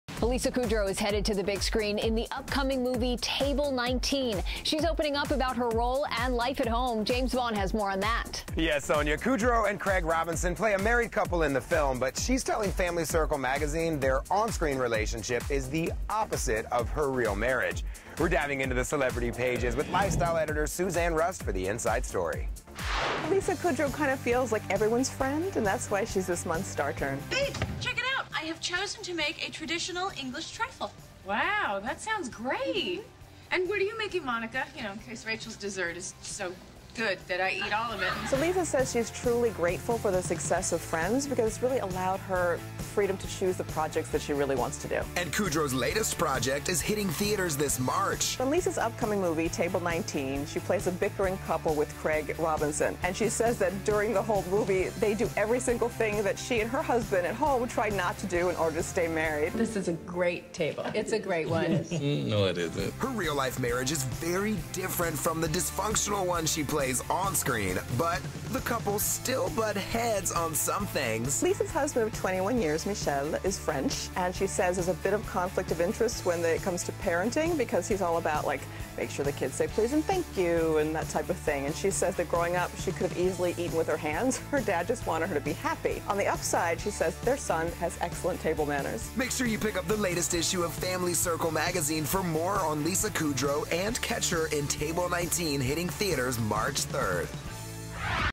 Family Circle Magazine: Lisa Kudrow
Family Circle Magazine's Lifestyle Editor Suzanne Rust is getting us the Inside Story on Lisa Kudrow.